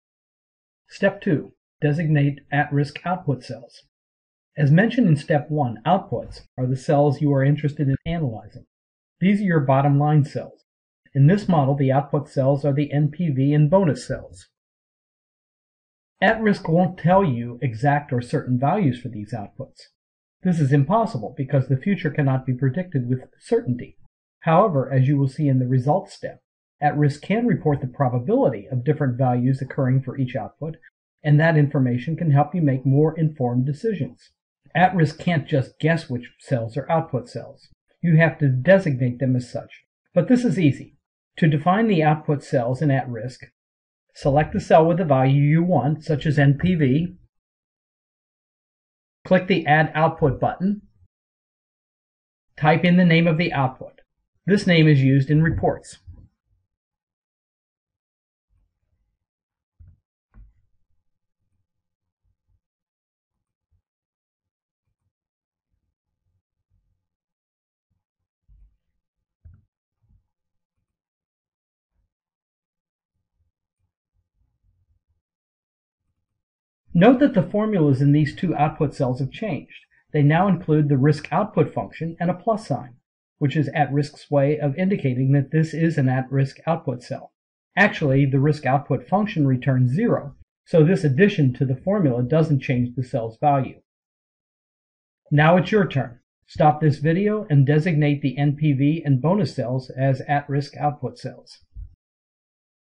QuickStart - Step 2: Add @RISK Outputs
This is 'Step 2: Add @RISK Outputs' of the interactive tutorial that is designed to teach you how to use @RISK software by walking you through an actual risk model.
The Quick Start model shown in the videos is bundled with the software. You can access the model by clicking on the Quick Start link shown in the software welcome screen at launch, or by selecting Example Spreadsheets from the software Help menu.